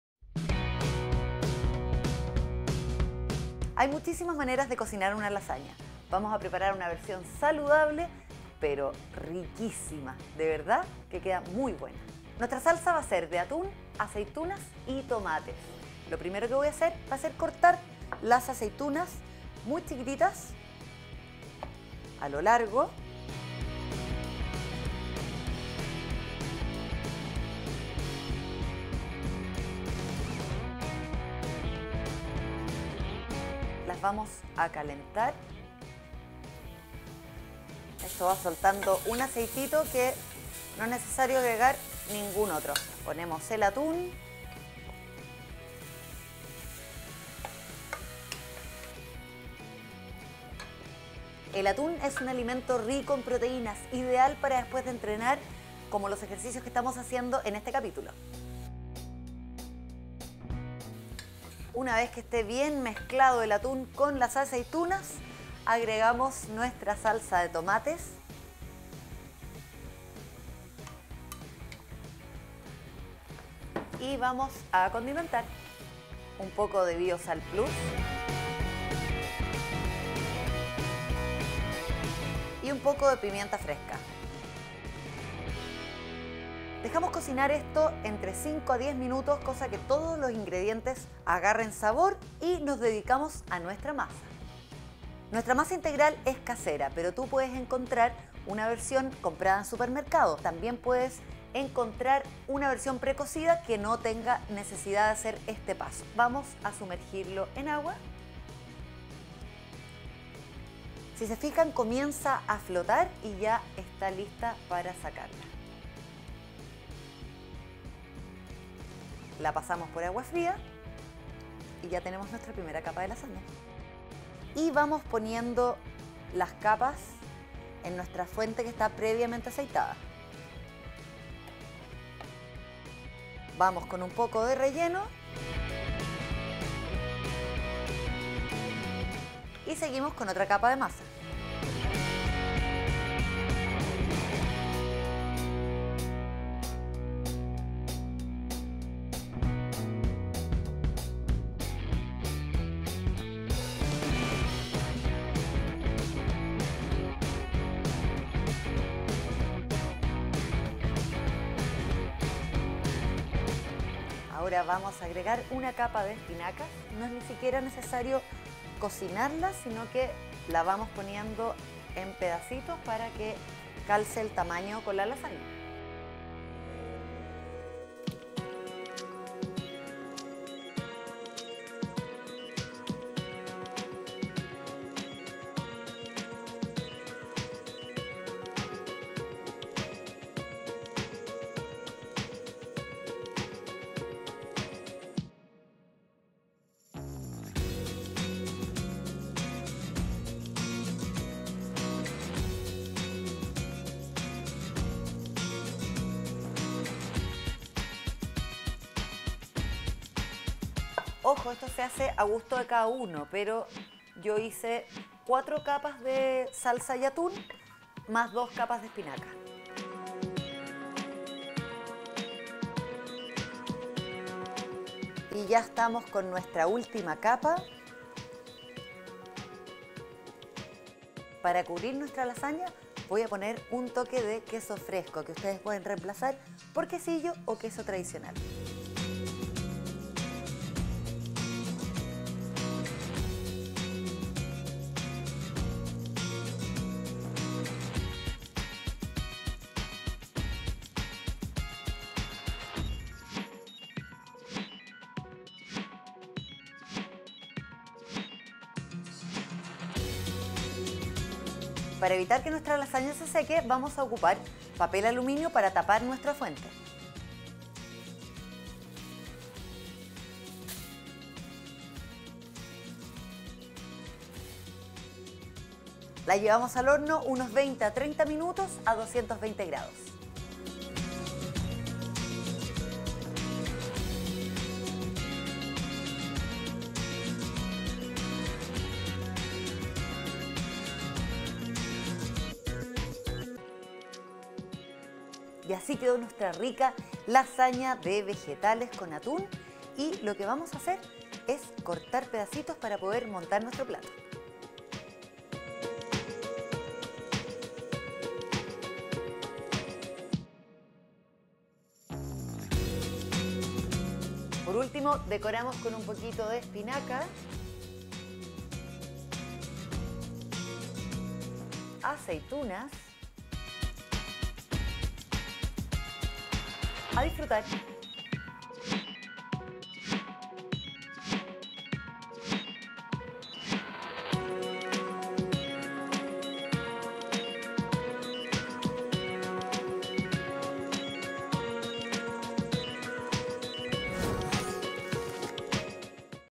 LASAÑA DE ATÚN Y ESPINACA / MANOS A LA OBRA
Disfruta de comer rico y saludable con esta lasaña espectacular!

INGREDIENTES:
- Masa de lasaña integral pre-cocida.
- Aceitunas verdes.
- 2 latas de atún.
- Salsa de tomate a gusto.
- 1 mata de espinacas.
- Queso fresco o quesillo.

PASO-PASO:

Cortamos las aceitunas en trozos alargados y calentamos en una olla, junto con las 2 latas de atún y salsa de tomate a gusto; mezclamos bien y condimentamos con Biosal Plus y pimienta. Una vez lista la salsa, remojamos la masa integral pre-cocida en una olla de agua caliente y luego en un recipiente de agua fría. Comenzamos a montar la lasaña: En una fuente con aceite poner la primera capa de masa integral, después la salsa, luego otra capa de masa y hojas de espinaca crudas. Este proceso se debe ir preparando de forma intercalada hasta lograr la altura deseada. Una vez terminada agregar queso fresco o quesillo en la parte superior y llevar a un horno de 220° por 20-30 minutos. Servir y decorar con espinaca y aceitunas.